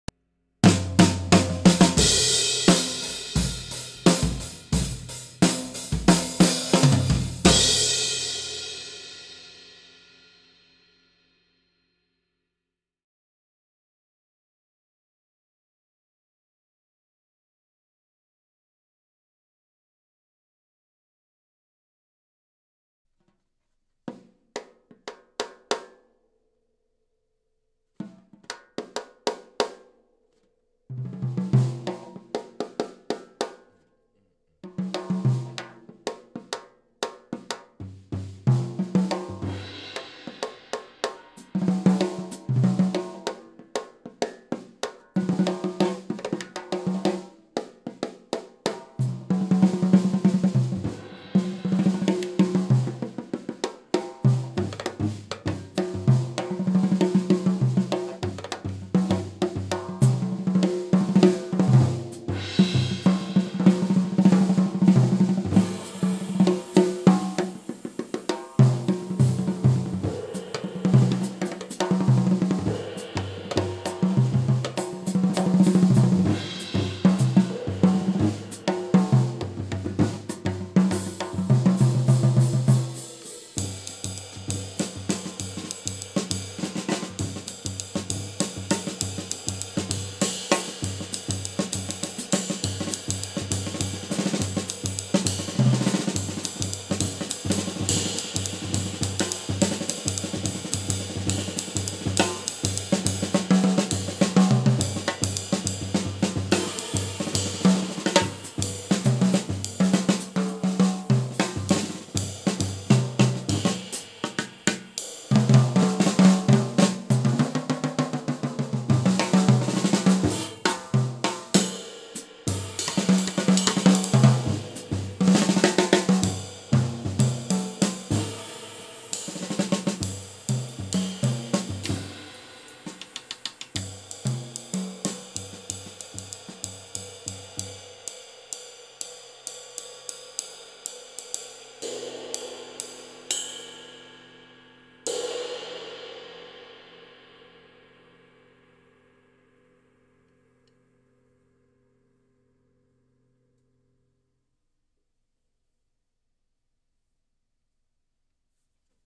Brooke Sofferman Plays His Sonor Drums & Agop Cymbals - Part 6
Gear list: Sonor SQ2 clear acrylic 14" x 18" bass drum, 8" x 12" rack tom, 14" x 14" floor tom, and 6" x 14" snare; Istanbul Agop 15" Traditional Medium Hi Hats, 20" Om Crash, 23" Matt Chamberlain Prototype Ride, and 18" Epoch Crash, and Hubback 4" ice bell.

Brooke endorses Sonor drums, Istanbul Agop cymbals, Pro-Mark sticks, and Tangereens percussion sticks.